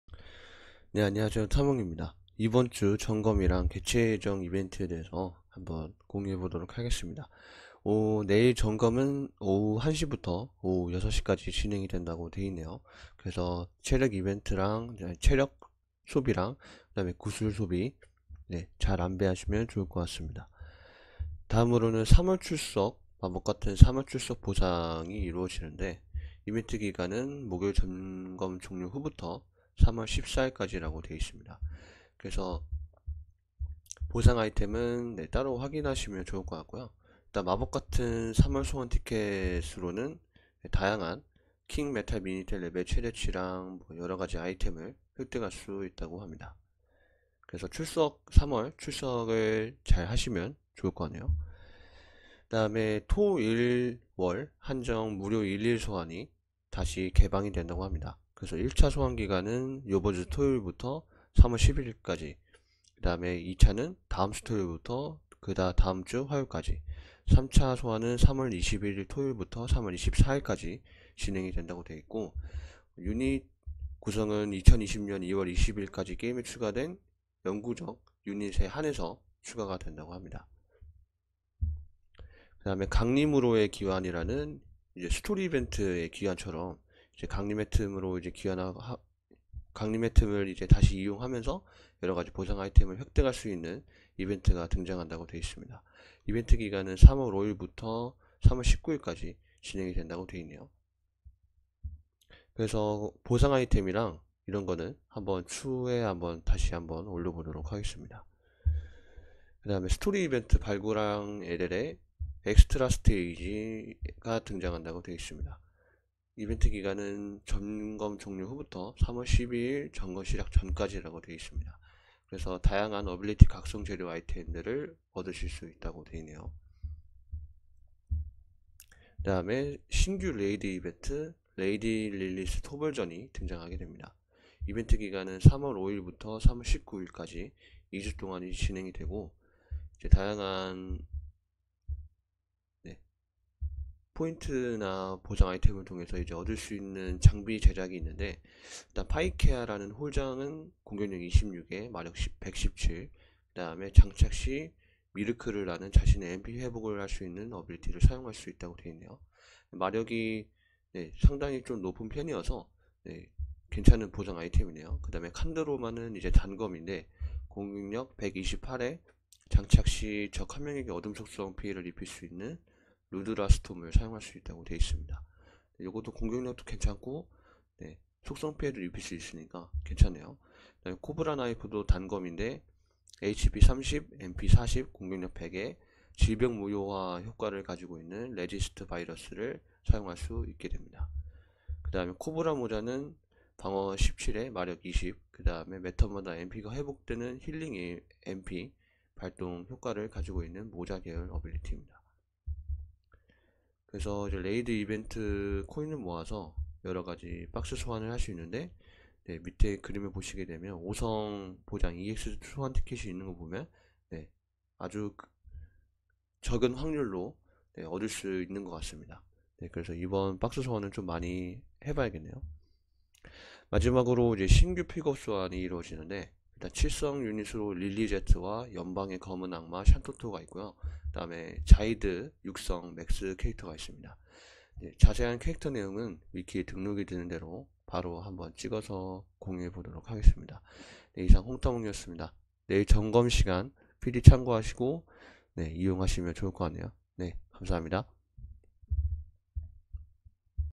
3월 5일 점검과 개최예정이벤트 안내
FFBE 3월 5일 정기점검 시간과
개최예정이벤트 안내